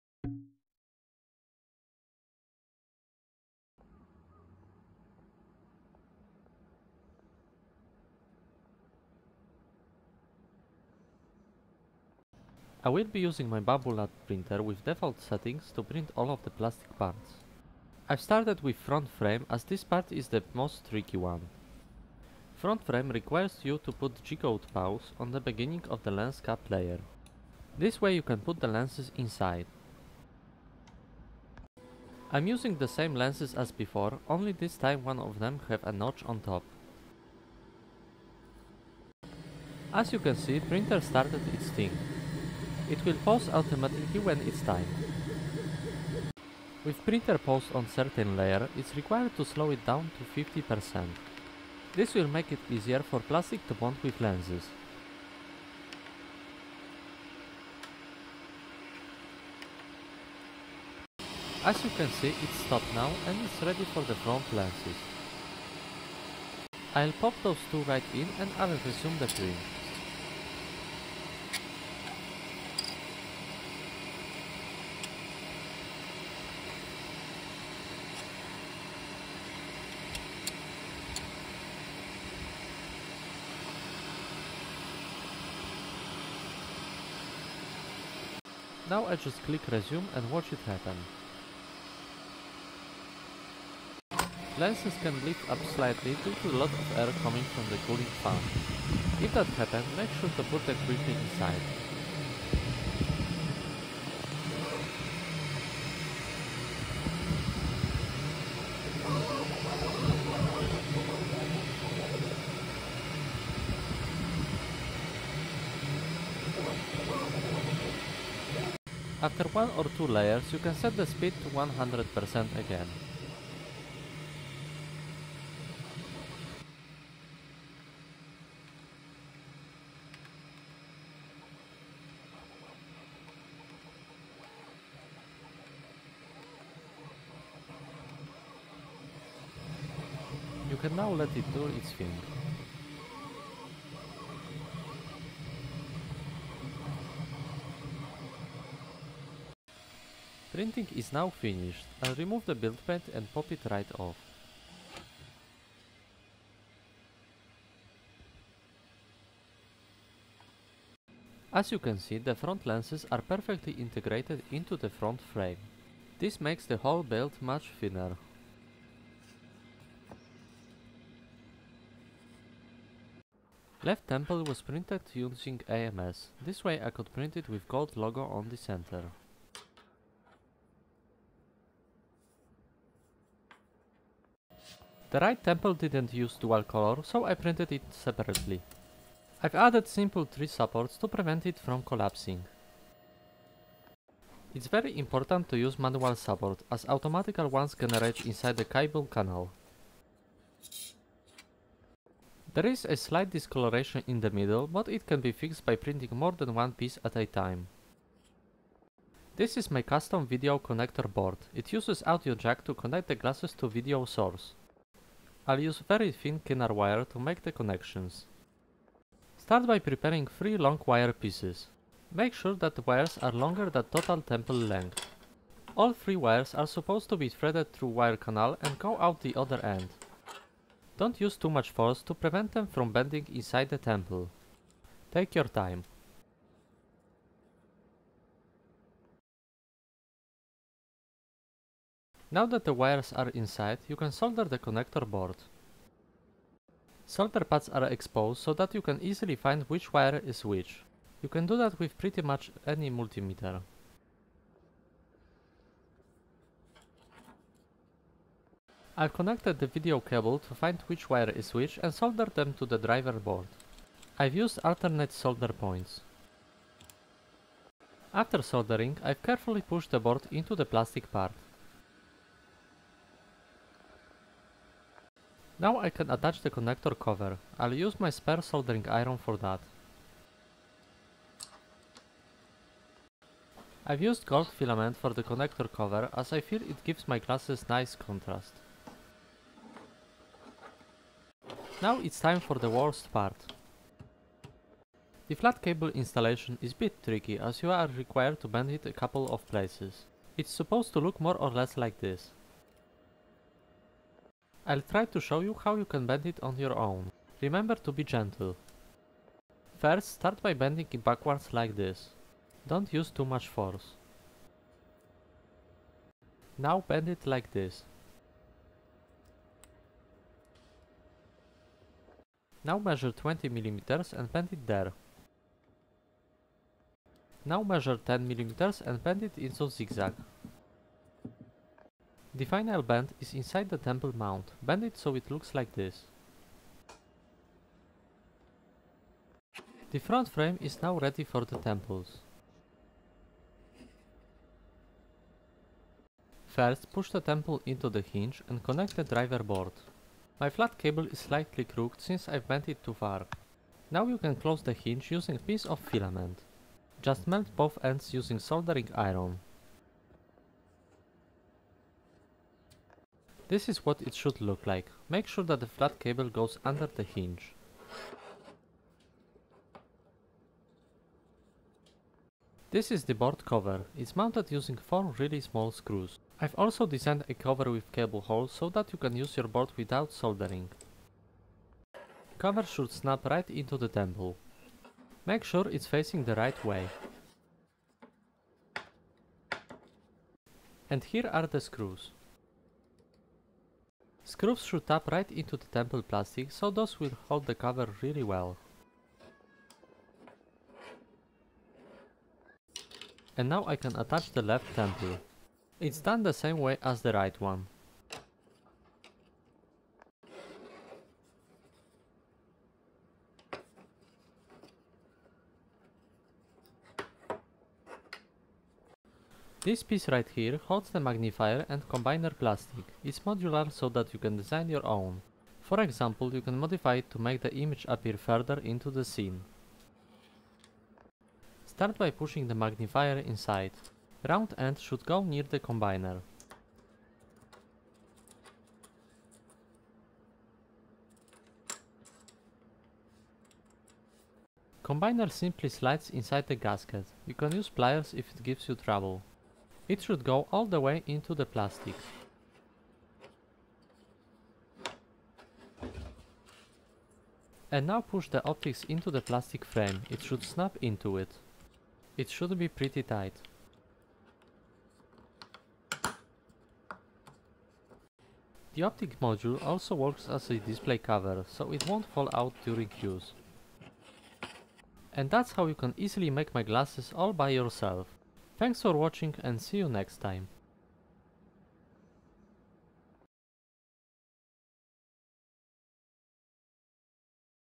Onyx Glasses 1.5 | Full DiY Assembly
This is my assembly video for those who want to make my glasses all by themself, without any Etsy kits.

Cut the combiner out of this: